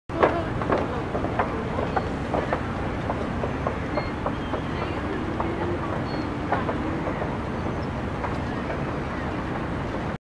941 Many yachts on dock
Many yachts on dock as they move along with the water and you can hear some birds making a noise near the port.